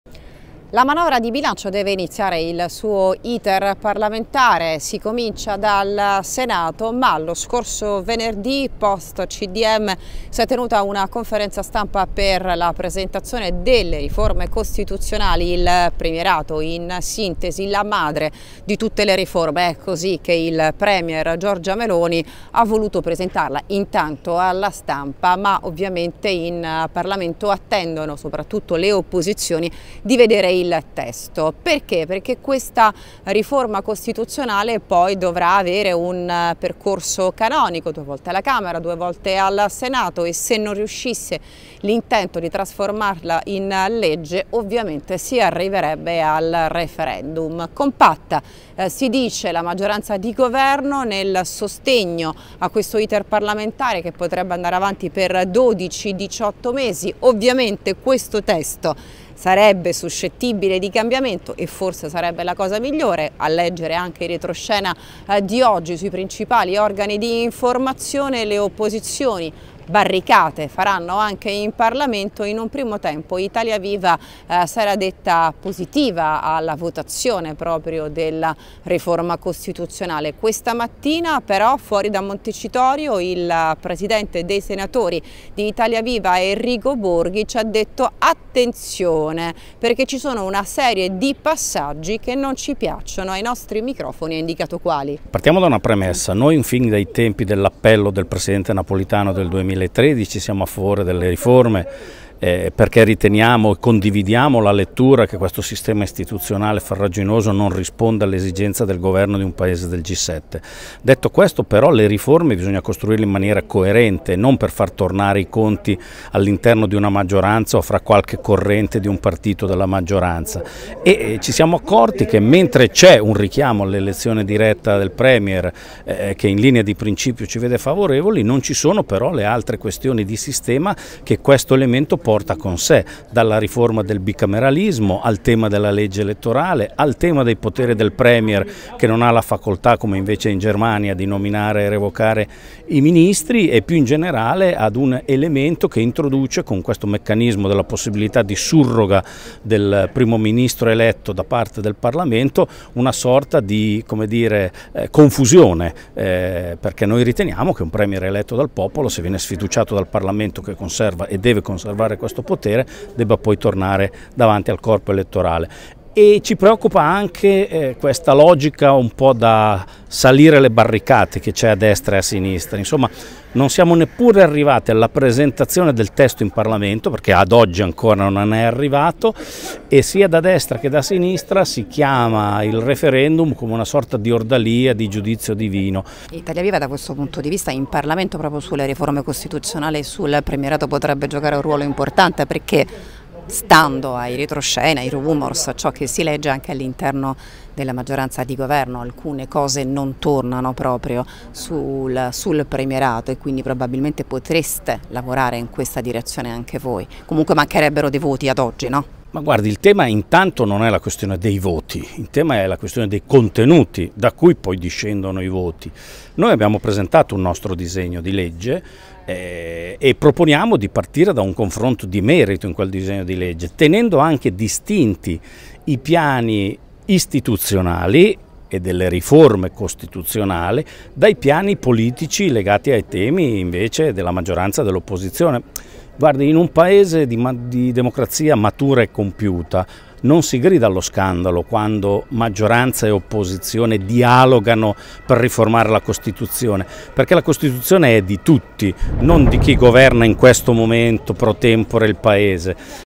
Premierato, Borghi a Notizie.com: “Questione di contenuti, proponiamo confronto di merito”
Premierato, le parole di Enrico Borghi capogruppo in Senato Italia Viva